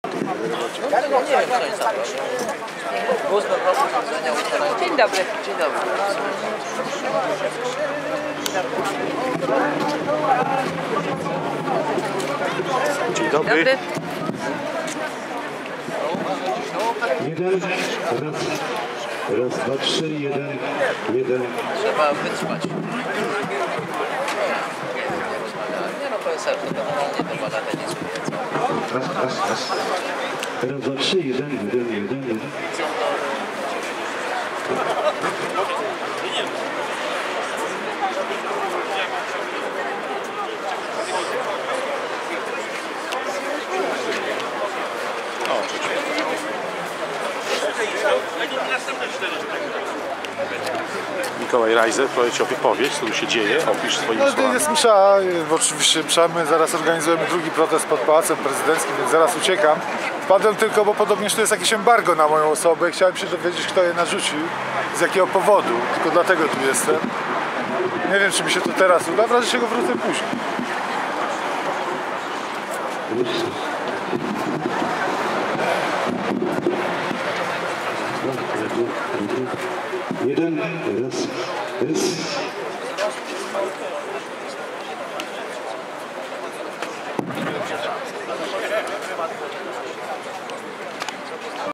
PROTEST ROLNIKÓW PRZED MSZĄ ŚWIĘTĄ 22-02-2015 MIKOŁAJ RAJZER PROTESTUJE
22 lutego 2015 - protest rolników polskich przed URM-em. Trwają przygotowania do Mszy Świętej. Przyszedł Mikołaj Rajzer (Warszawa Ursus) i również protestuje.

00507